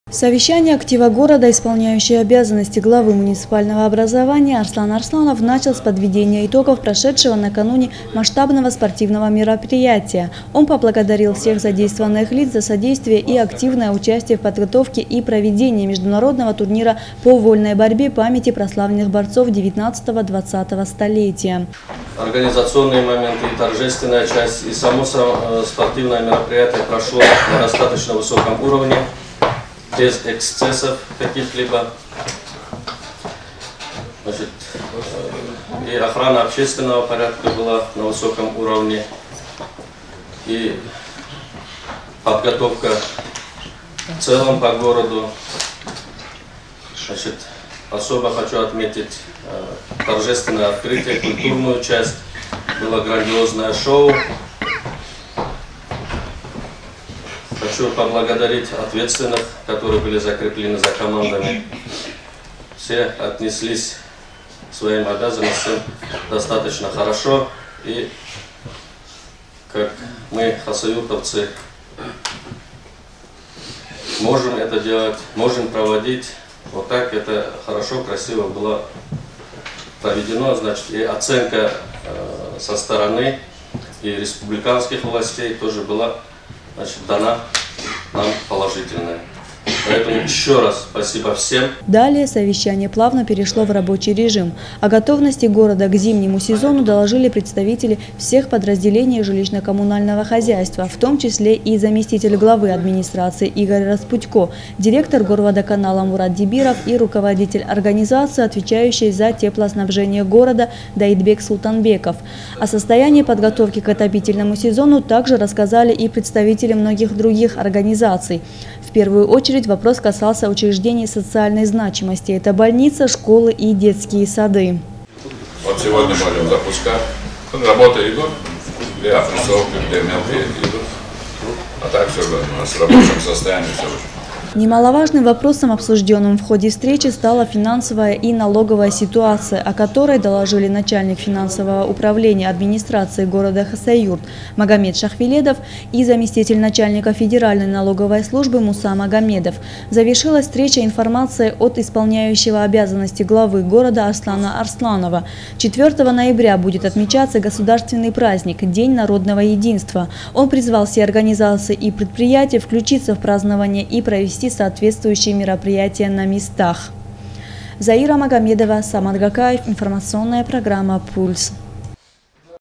Еженедельное аппаратное совещание актива города
В зале администрации состоялось очередное аппаратное совещание актива города, собравшее руководителей всех предприятий, организаций и ведомств муниципальных и государственных структур. В ходе встречи были затронуты наиболее интересные и волнующие вопросы, касающиеся отдельно определенных отраслей и города в целом.